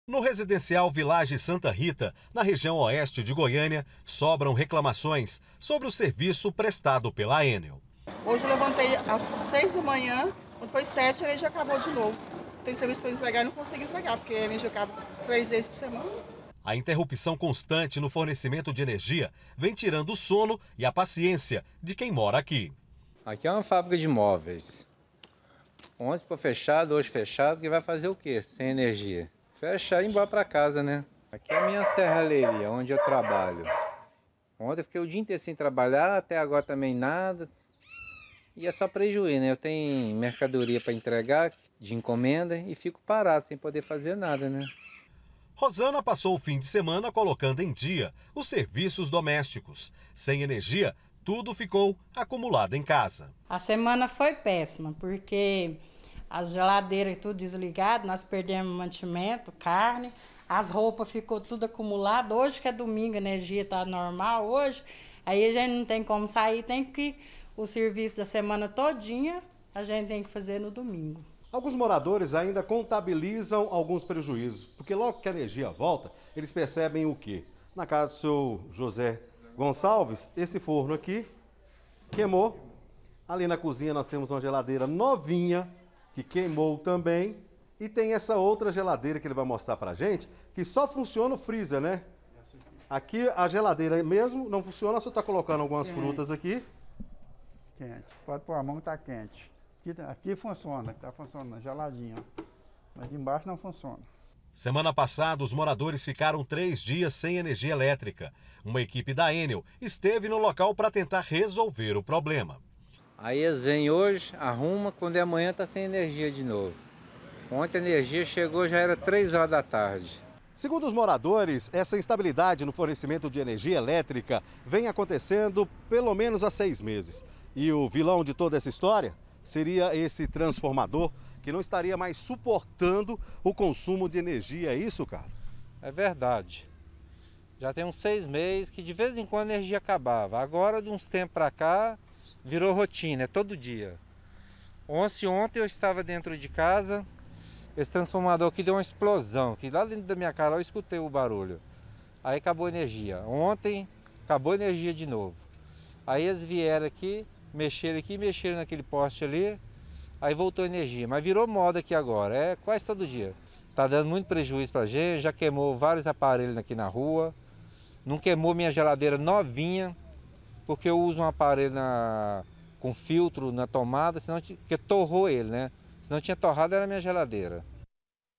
PREJUÍZOS: MORADORES SOFREM COM A FALTA DE ENERGIA
E A SITUAÇÃO DOS MORADORES DO RESIDENCIAL VILLAGE SANTA RITA NÃO ESTÁ NADA FÁCIL. ELES ESTÃO SOFRENDO COM A FALTA DE ENERGIA NA REGIÃO QUE ESTÁ ACONTECENDO QUASE TODOS OS DIAS. GELADEIRAS, FORNOS ENTRE OUTROS ELETRÔNICOS FORAM QUEIMADOS E ACABOU TRAZENDO UM GRANDE PREJUÍZO AOS MORADORES.